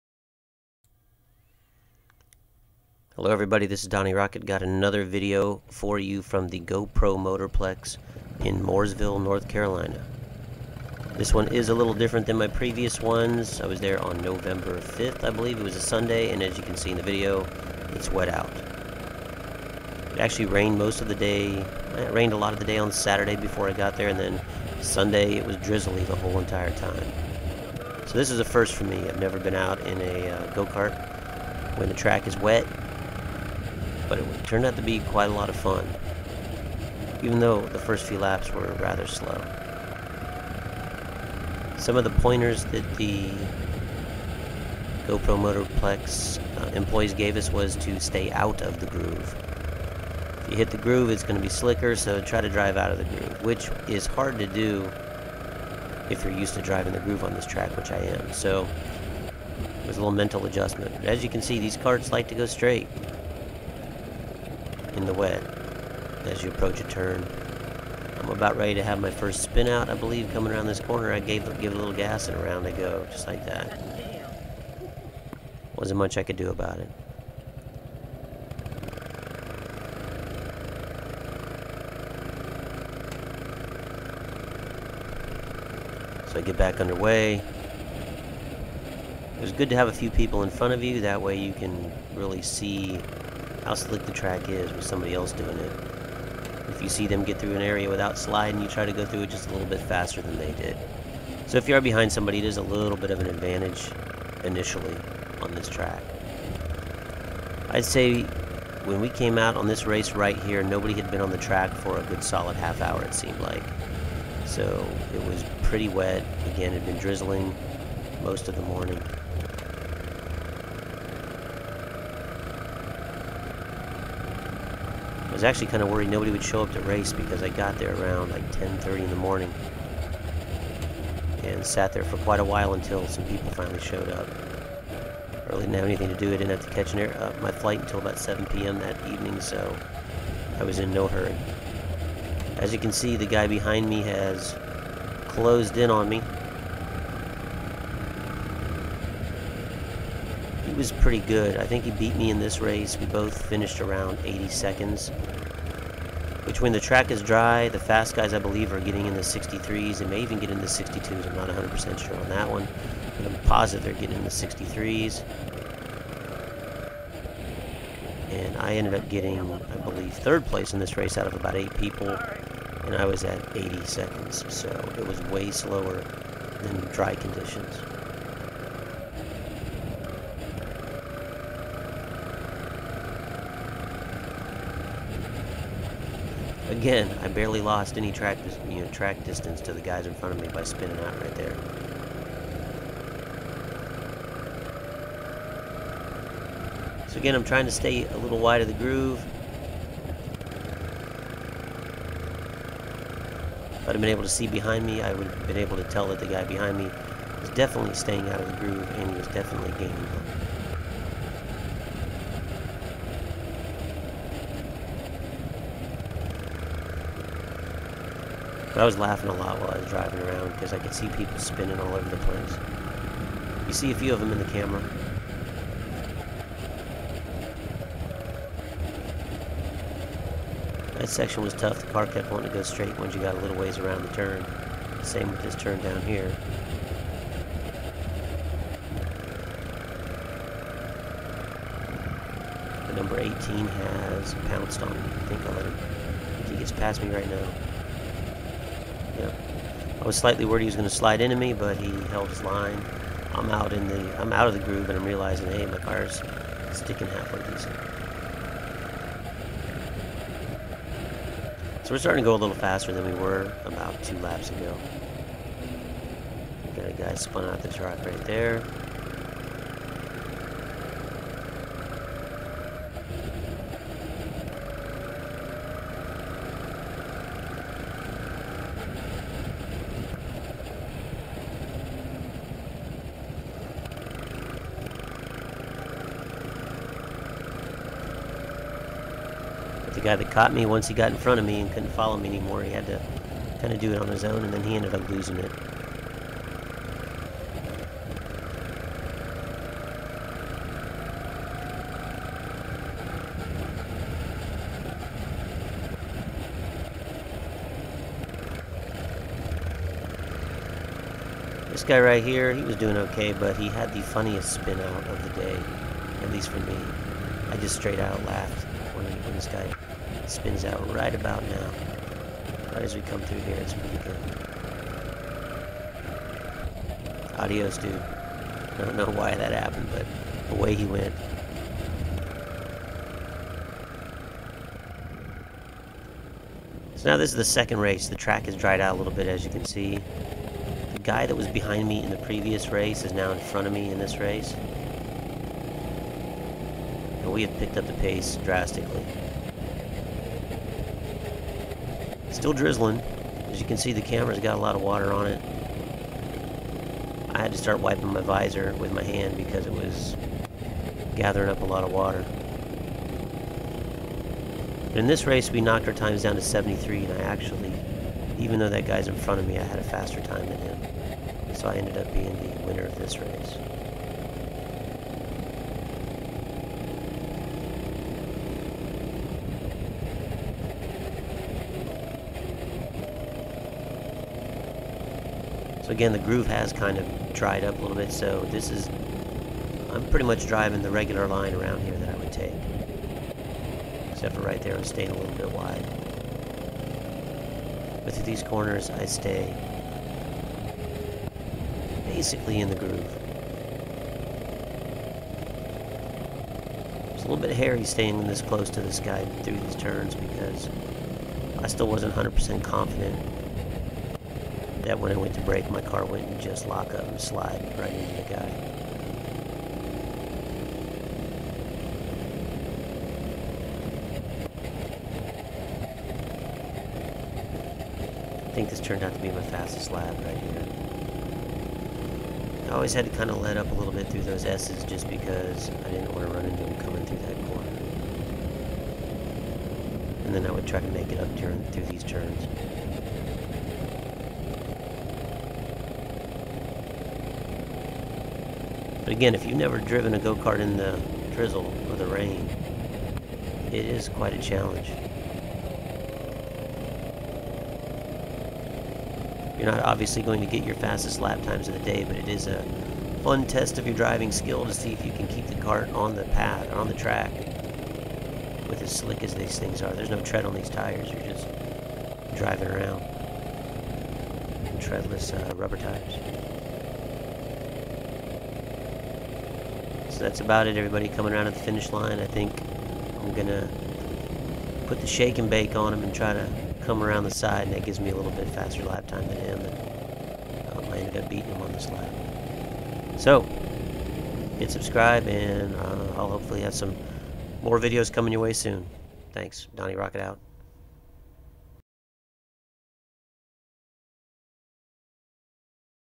GoPro Motorplex, in the drizzle ..... my nizzle.
Some first hand footage of what it's like to drive a CRG go-kart in the drizzle.  Super slick at first, but still a lot of fun.  Laps in the vid start slow, but after a few races the groove dried out a bit, and times improved.